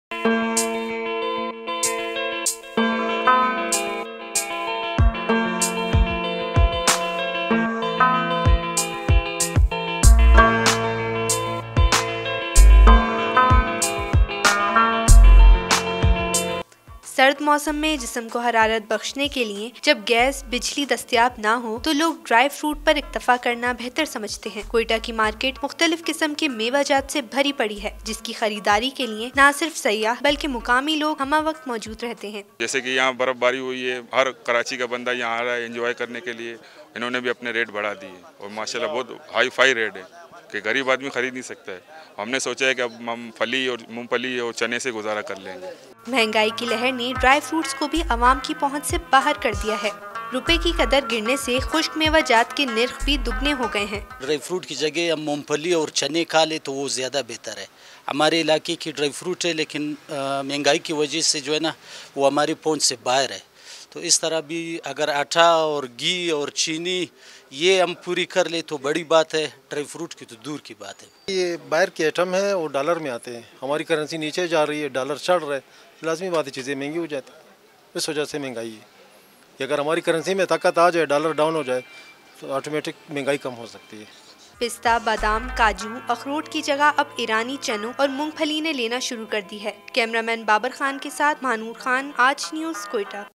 Wave of inflation also made dry fruits out of reach of people | Aaj News
Aaj TV Official:
shorturl.at/iBDNX